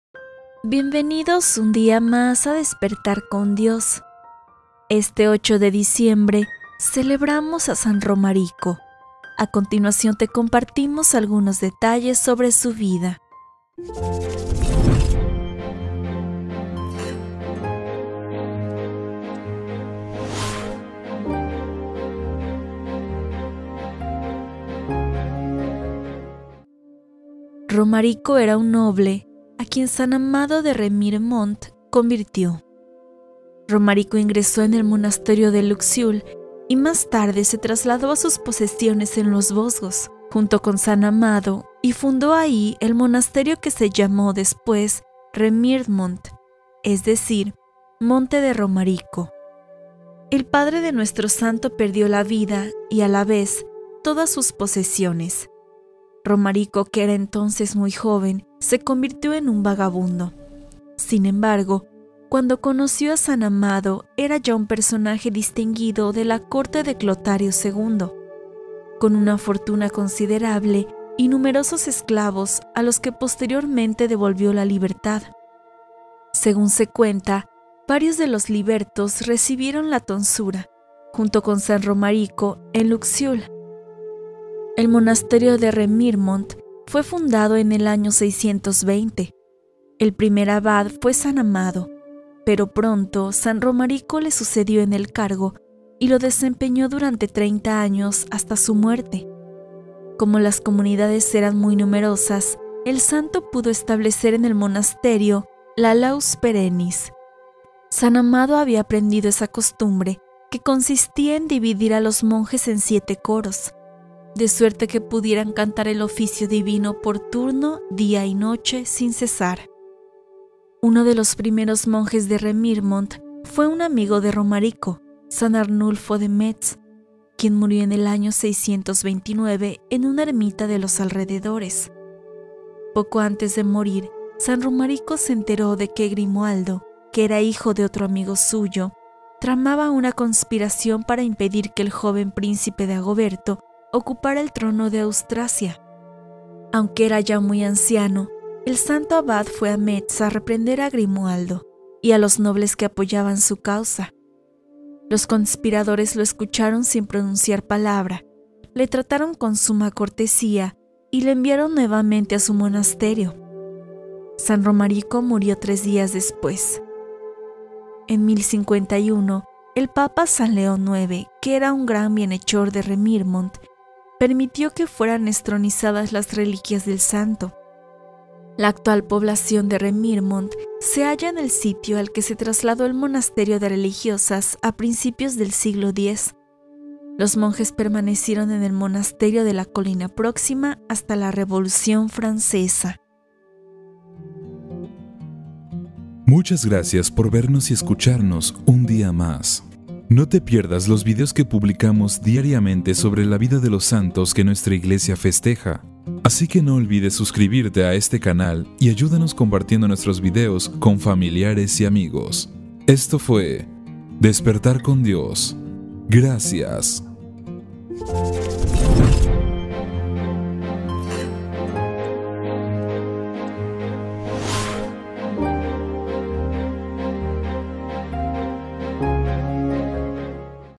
Quién Fue San Romarico | Santo del Día 8 de Diciembre de 2020
Te compartimos la biografía de San Romarico